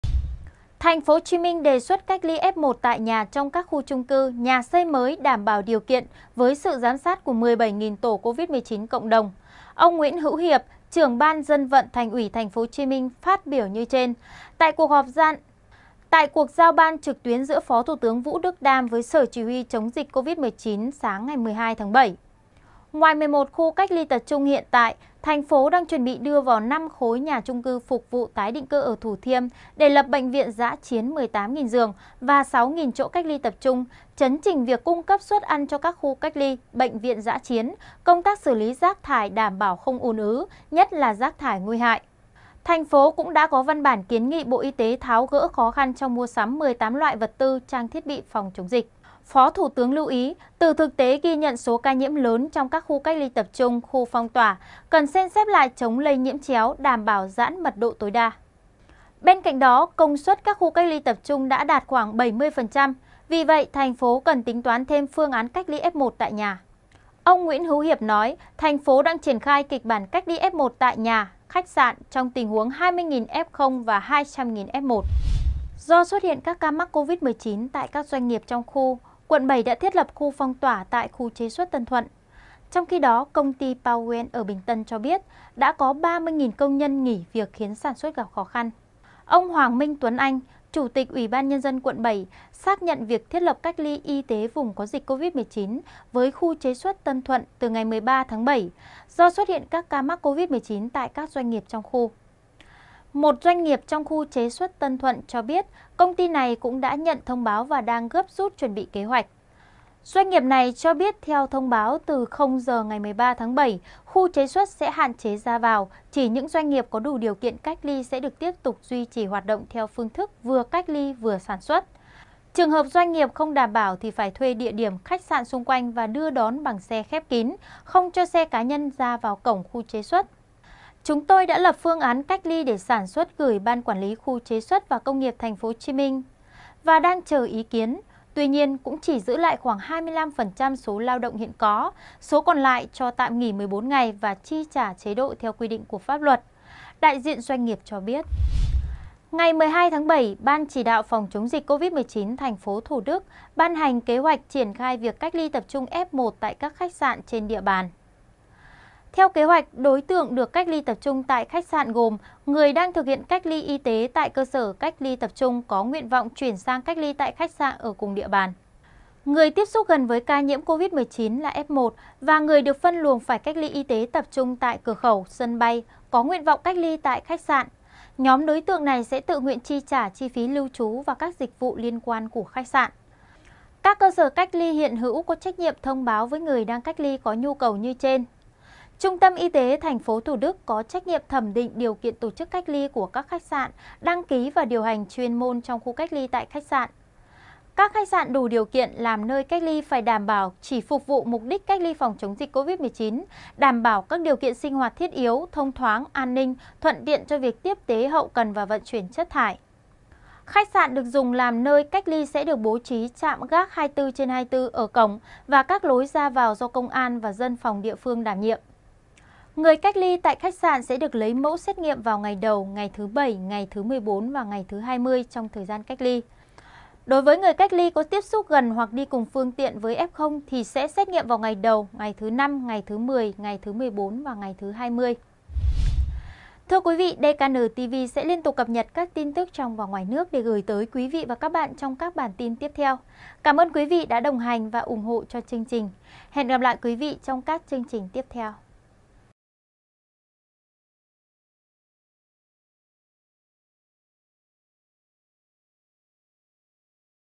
TP HCM kiến nghị mở rộng cách ly F1 tại nhà
- Thiết lập phong tỏa Khu chế xuất Tân Thuận; PouYuen 30.000 công nhân nghỉ việc tránh dịch
- Thủ Đức chuẩn bị cách ly tập trung F1 tại khách sạn

DKN News kính chúc quý độc giả mọi điều tốt lành.